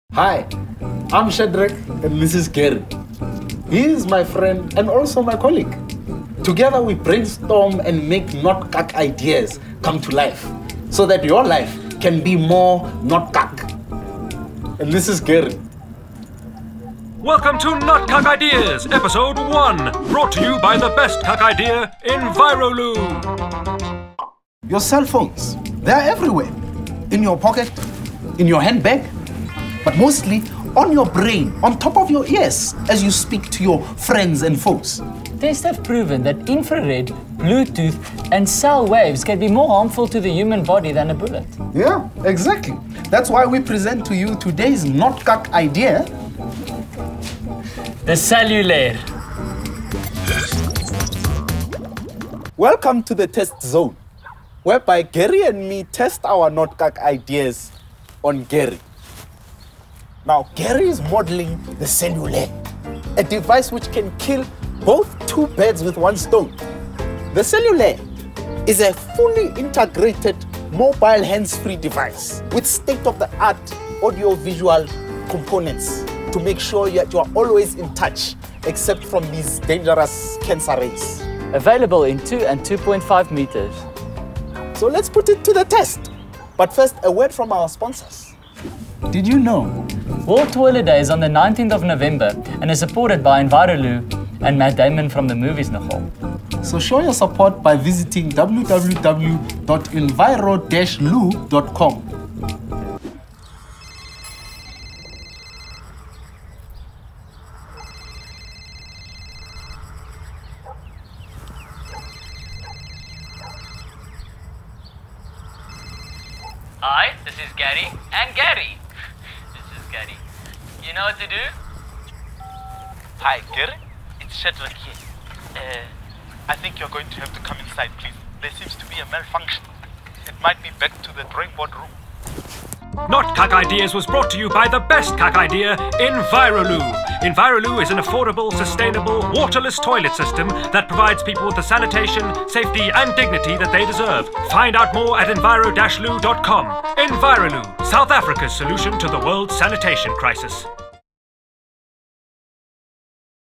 Enviro Loo Cellulair
Enviro Loo is an affordable, sustainable waterless-toilet system that provides millions of people with the sanitation, safety and dignity that they deserve. Enviro Loo South Africa's solution to the world's sanitation crisis.